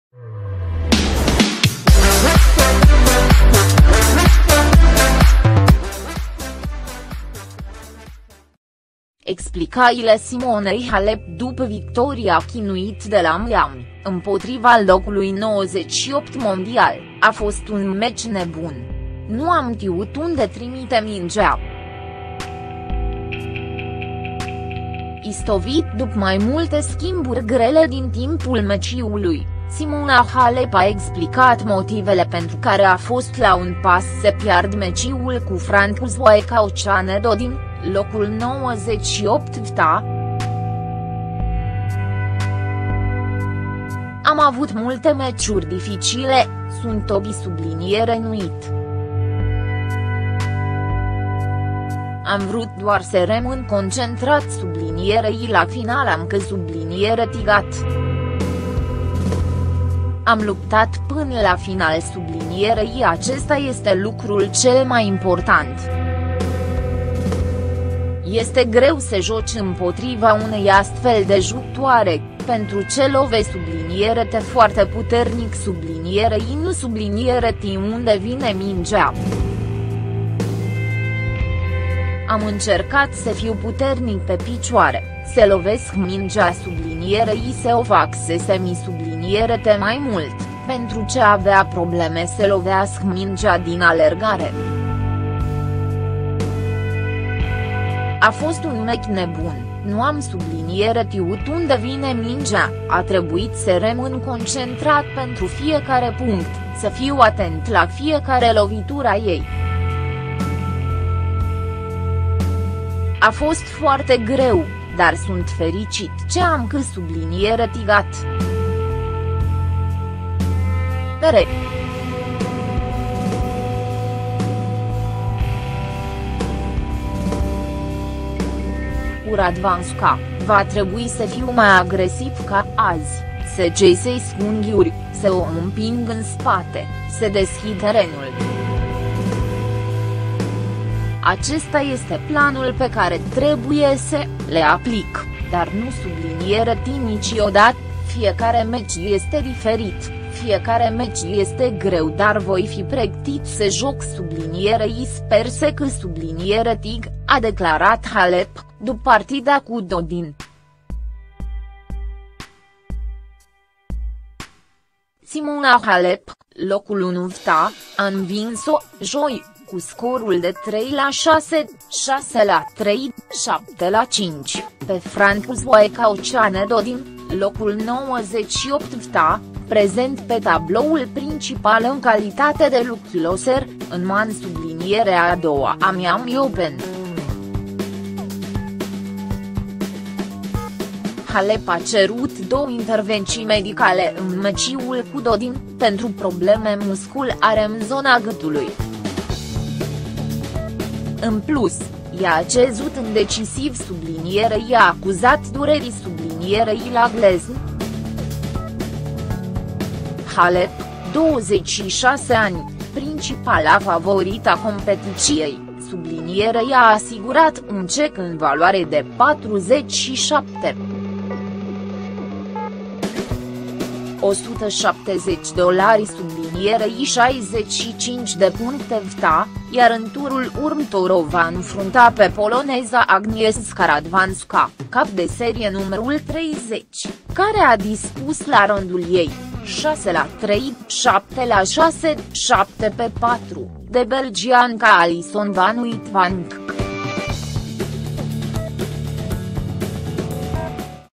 Explicațiile Simonei Halep după victoria chinuită de la Miami, împotriva locului 98 mondial
Explicațiile Simonei Halep după victoria chinuită de la Miami, împotriva locului 98 mondial: 'A fost un meci nebun. Nu am știut unde trimite mingea'